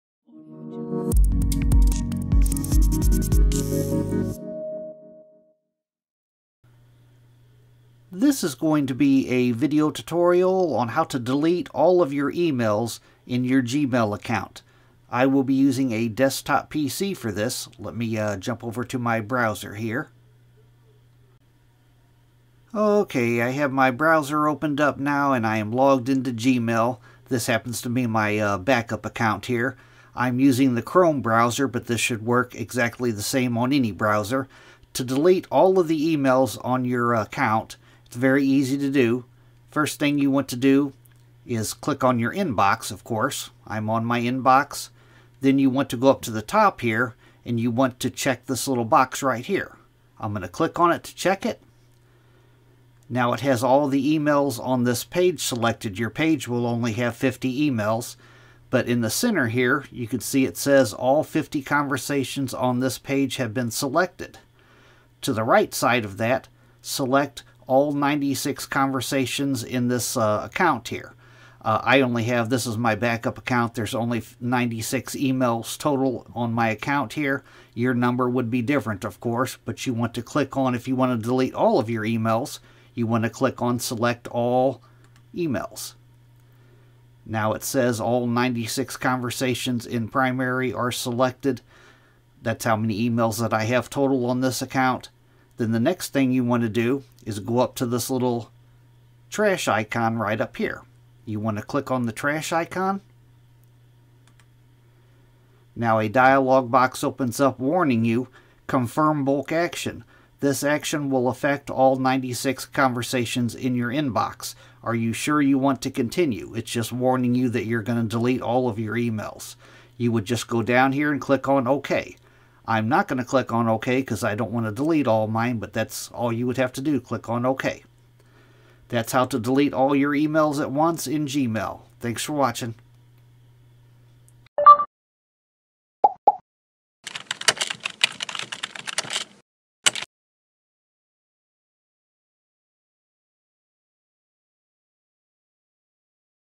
How To Delete All Emails At Once In Gmail | PC Tutorial
Ready to finally purge your Gmail inbox? 🧹 This quick PC tutorial will show you exactly how to delete all your emails at once in Gmail. Whether you're starting fresh or clearing out thousands of old messages, this step-by-step guide gets the job done with just a few clicks.

Simple Steps
1. Open Gmail in your browser.
2. Click on the "Selection" box just above the emails.
3. Click on "Select All Conversations In The Inbox".
4. Click on the "Mark As Read" icon.
5. In the confirm bulk action dialog box click "OK".

Chapters
0:00 How To Delete All Emails At The Same Time In Gmail
0:20 Sign Into Gmail
0:35 Make Sure You Are On The Inbox Tab
0:45 Click The Selection Box To Check It
1:00 Click On Select All Conversations In The Inbox
1:35 Click On The Trash Can Icon
1:45 Confirm Bulk Action